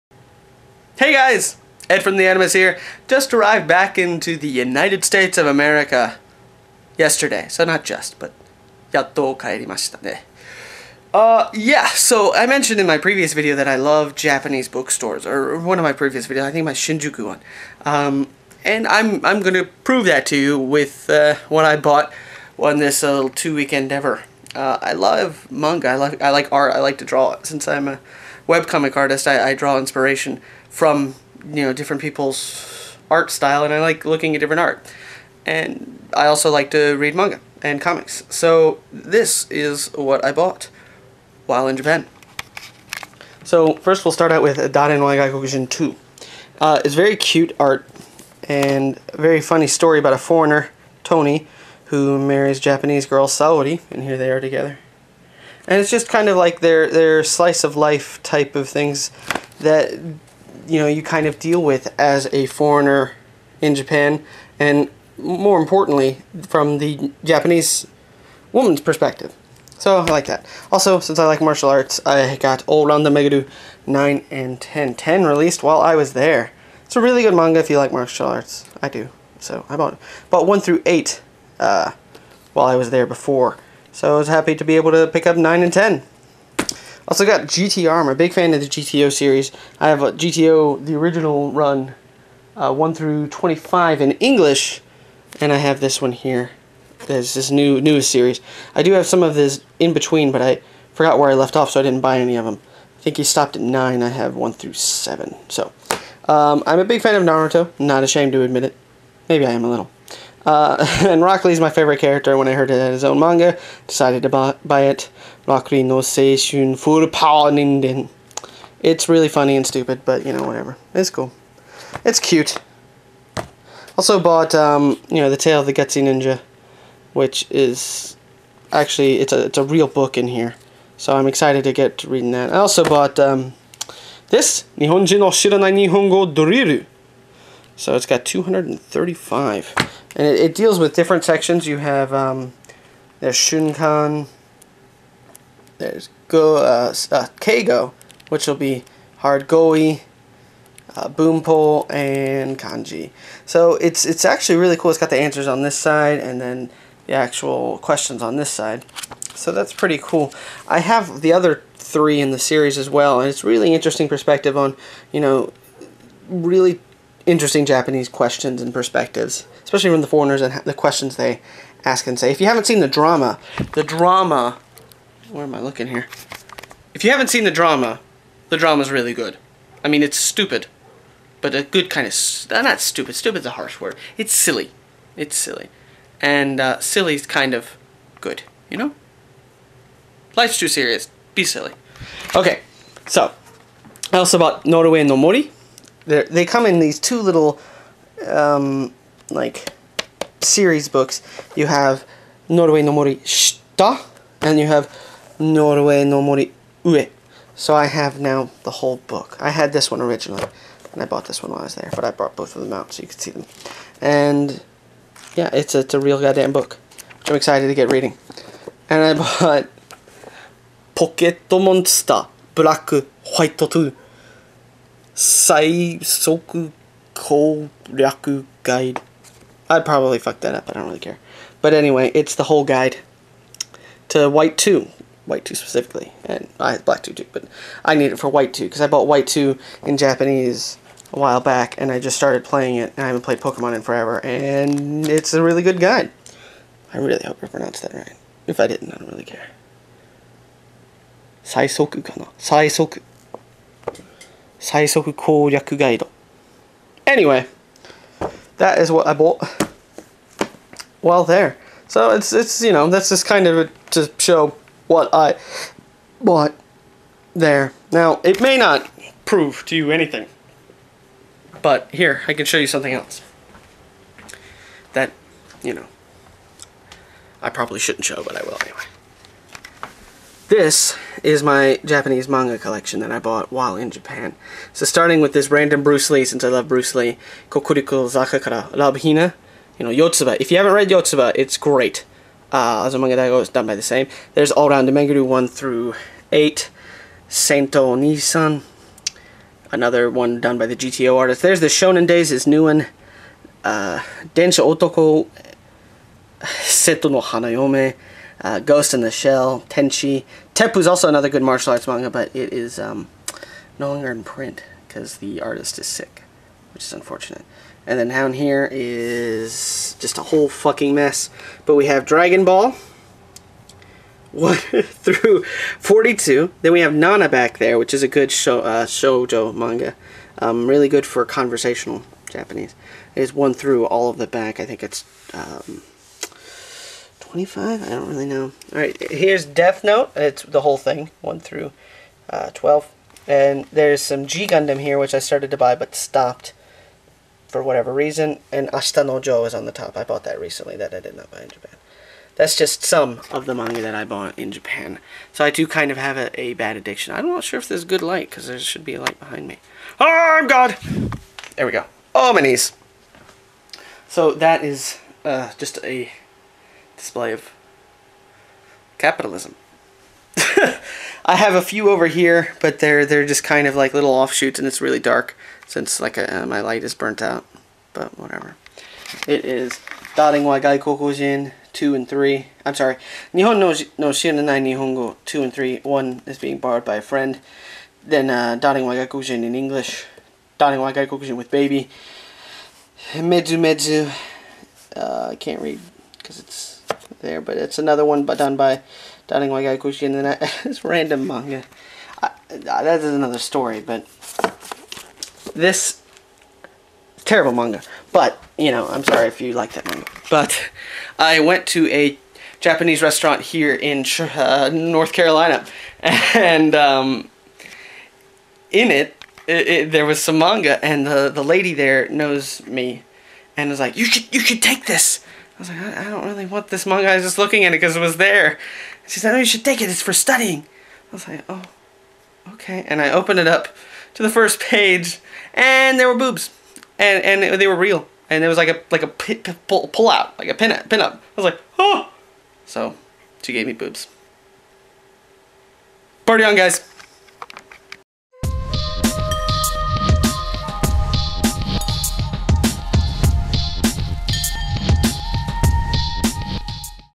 Japanese Book Addiction
I like manga and Japanese bookstores a bit too much. Oh, well...